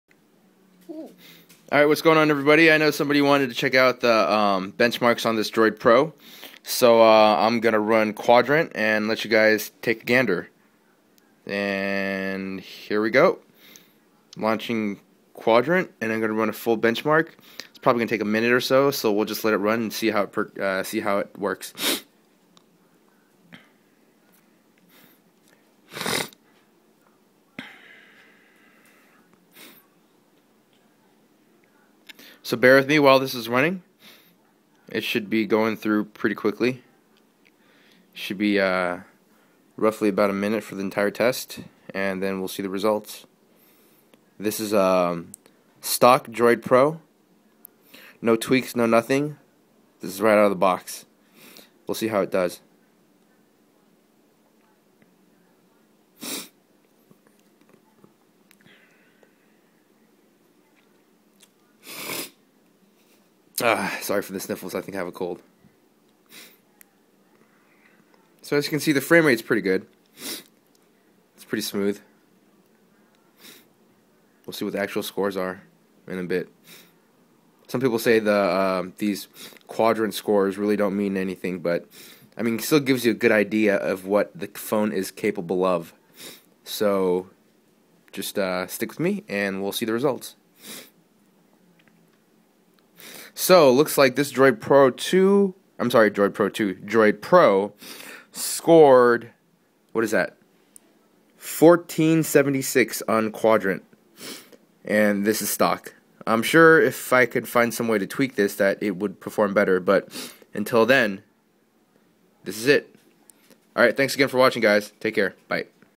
Droid Pro quadrant benchmarks
Stock Droid Pro running quadrant for benchmarks